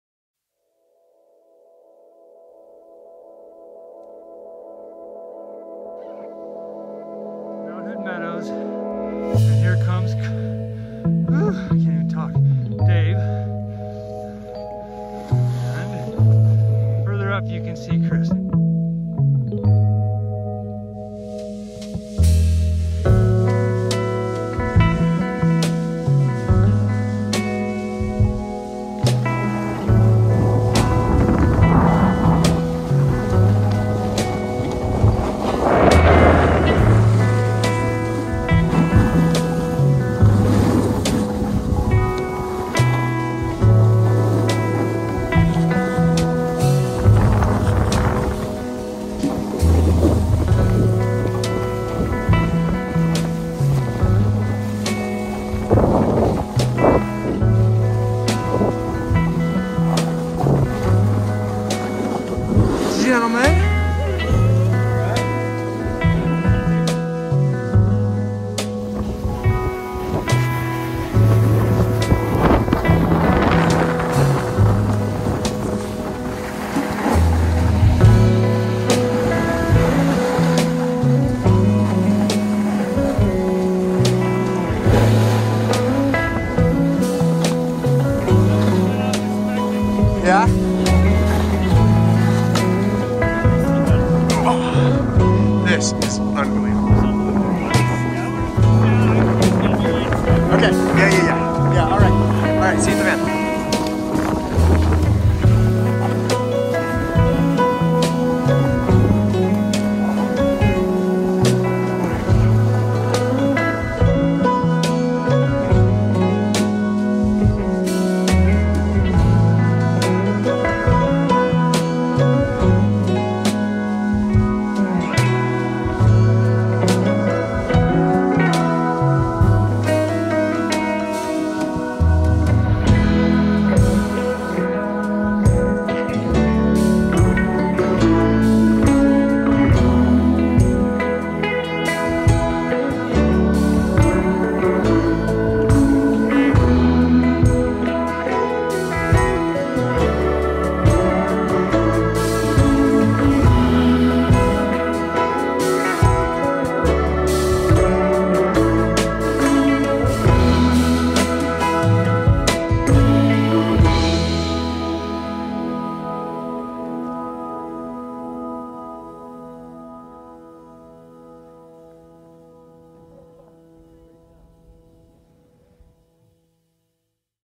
Skiing Mt Hood Meadows
Dave, Chris & Troy go skiing at Mt Hood Meadows, Dec 12th, 2022, and get a surprise bluebird day, above the clouds, while Portland remains overcast the whole day.